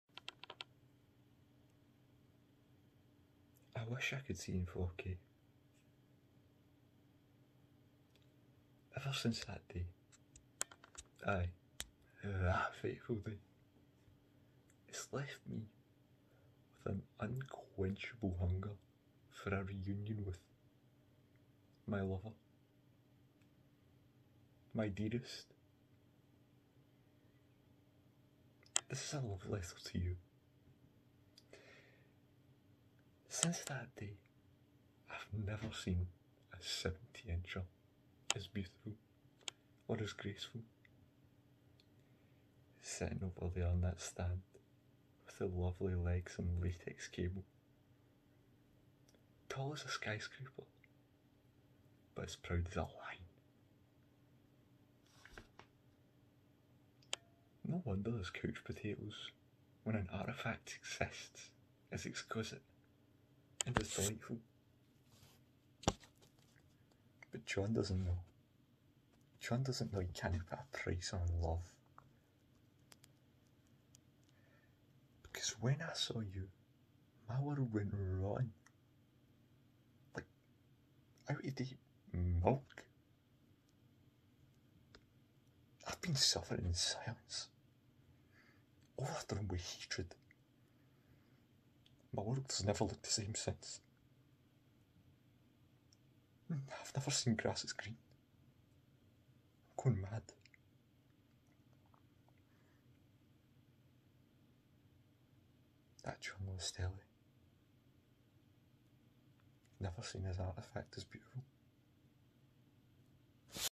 "Aht John Lewis Telly"
A short story about a man and his desire for the John Lewis telly. The man behind the video is none other than Mink (channel